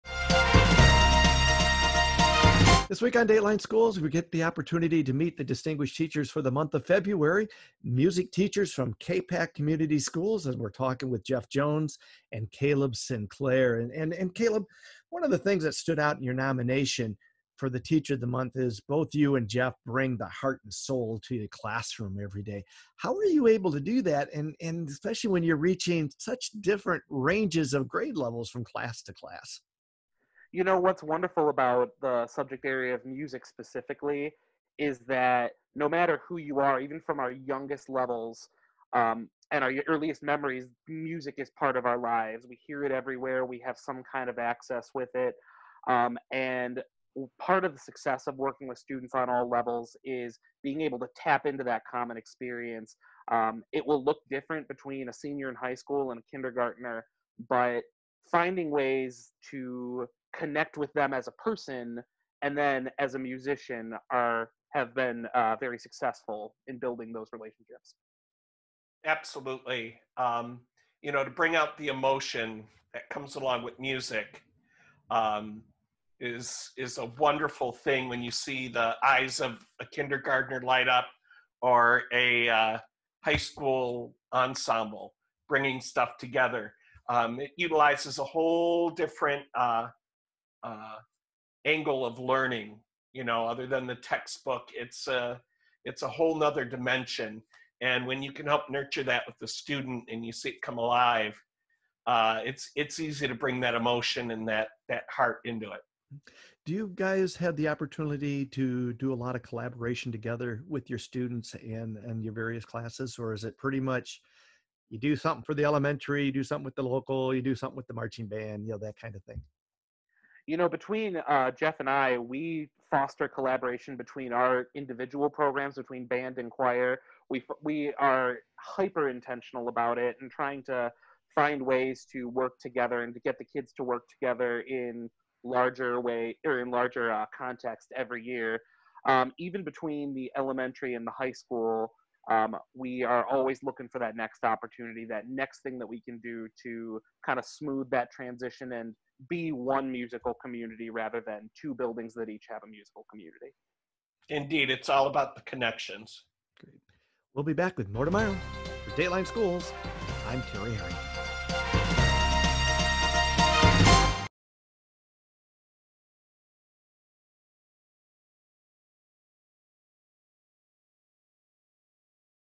Dateline Schools Distinguished Teacher of the Month February Wednesday
This week on Dateline Schools we talk to Distinguished Teachers of the Month for February Caleb Sinclair and Jeff Jones about the importance of music even with school on pause.